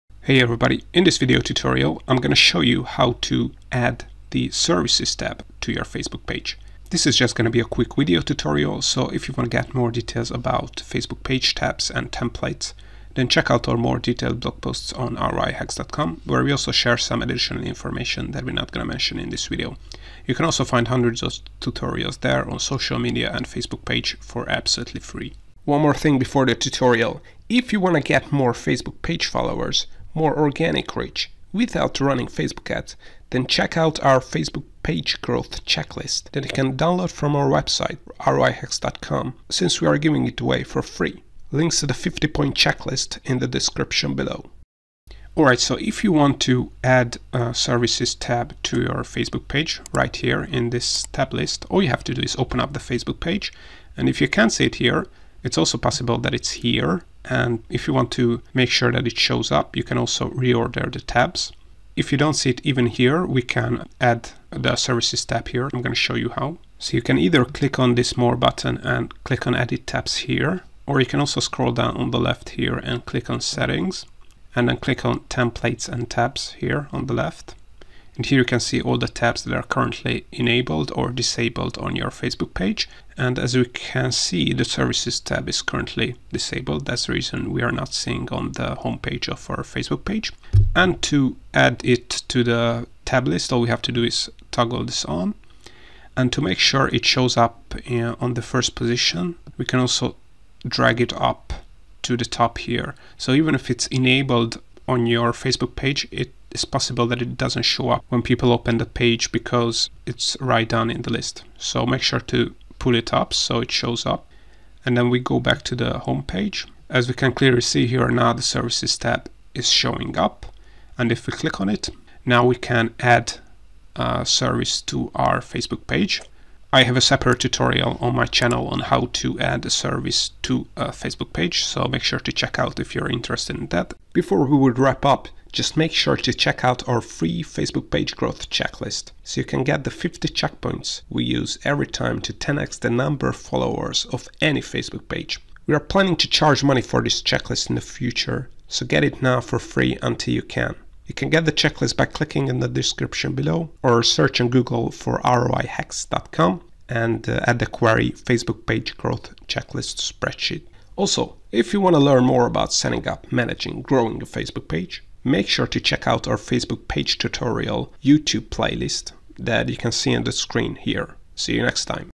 How To Add The Services Tab To The Facebook Page? [in 2025]
- 🚀 𝐆𝐫𝐨𝐰 𝐘𝐨𝐮𝐫 𝐁𝐮𝐬𝐢𝐧𝐞𝐬𝐬 𝐖𝐢𝐭𝐡 𝐅𝐫𝐞𝐞 𝐒𝐄𝐎 𝐓𝐫𝐚𝐟𝐟𝐢𝐜 𝐔𝐬𝐢𝐧𝐠 𝐒𝐄𝐌𝐑𝐮𝐬𝐡. 𝐒𝐭𝐚𝐫𝐭 𝐔𝐬𝐢𝐧𝐠 𝐒𝐞𝐦𝐑𝐮𝐬𝐡 𝐅𝐨𝐫 𝐅𝐫𝐞𝐞
👉 𝐒𝐜𝐡𝐞𝐝𝐮𝐥𝐞 𝐘𝐨𝐮𝐫 𝐒𝐨𝐜𝐢𝐚𝐥 𝐌𝐞𝐝𝐢𝐚 𝐏𝐨𝐬𝐭𝐬 𝐅𝐨𝐫 𝐅𝐫𝐞𝐞

✅How to Add The Services Tab To Your Facebook Business Page?  - Complete blog post

In this video tutorial, we are going to show you Add The Services tab To Your Facebook Page.

How To Add Services Tab To A Facebook Page?
Here is how to add the Jobs tab to the Facebook page:

Open the Facebook page
On the left column, click on ‘Settings’
Then again on the left, click on ‘Templates and Tabs’.
Scroll down here and enable the Facebook ‘Services’ tab.how to add services tab to a Facebook page
Now, when you go back to the home page of your Facebook page, you should be able to see the Services tab now showing up there.

Services Tab Missing From The Facebook Page
If your Services Tab is still missing from the Facebook page, the most likely reason that is hidden from the top row.

Facebook only shows the first couple of your Facebook page tabs, so you might have to click on the down arrow in the tab list to be able to see the Services tab.

In this case, you can reorder your Facebook page tab list, so you can make sure that the necessary tabs show up without clicking on the down arrow.

Can’t Add Services Tab To Facebook Page
If you can’t add the Services tab to the Facebook page, the most likely reason is that you don’t have admin access on the Facebook page.

Here are some useful tutorials on how to add yourself as an admin to the Facebook page:

How to add admin to a Facebook Page
Request admin access to a Facebook page
How To Remove The Services Tab From Facebook Page?
If you no longer need the Services tab on your Facebook page, you can easily remove the tab from the Facebook page:

Scroll down here and disable ‘Services’ tab of the Facebook page